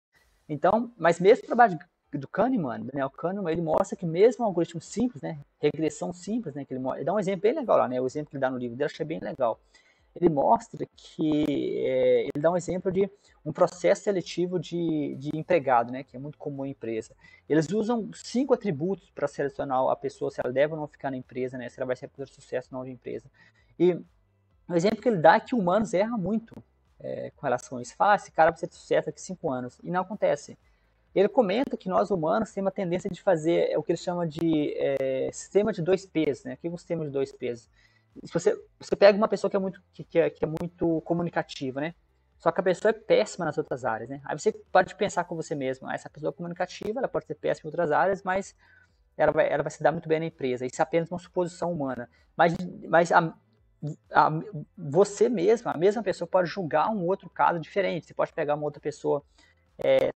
Um sistema de dois pesos: como humanos fazem uma lambança na hora de decidir
Nota importante. esse título é provocativo! Não há generalizações para todos os cenários!

Contudo, os resultados ainda são assustadores. Como ele mesmo comenta, nós não estamos prontos a deixarem os modelos decidirem, isso seria um atributo humano. No pior cenário, os modelos empatam com humanos, que já é surpreendente, especialmente porque não são voláteis e susceptíveis a fatores externos, como humor.

Uma pessoa reagiu no Facebook, não seria um medo de modelos???🤔🤔

 Kahneman comenta isso no livro.
Temos discussões no nosso livro, talvez saia um artigo!

"“In one of the three samples, 77% of the ten thousand randomly weighted linear models did better than the human experts. In the other two samples, 100% of the random models outperformed the humans.” Cass R. Sunstein, Daniel Kahneman, and Olivier Sibony"